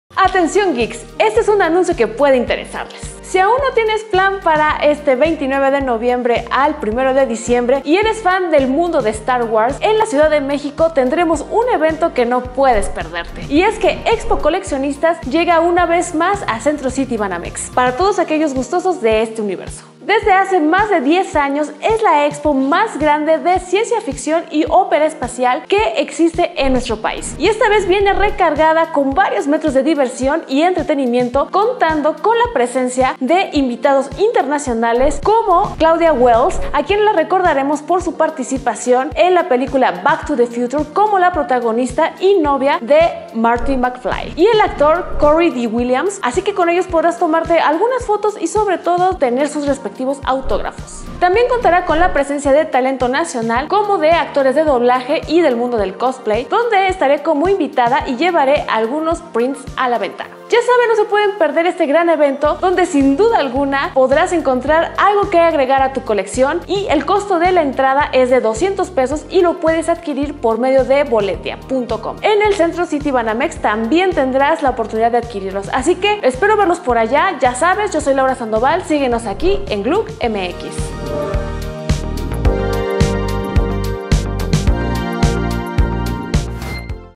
EXPO COLECCIONISTAS 2019
¡Atención geeks! Este fin de semana no te puedes perder la 'Expo Coleccionistas 2019'. Esto es todo lo que debes saber sobre este evento.

Por Laura Sandoval 📺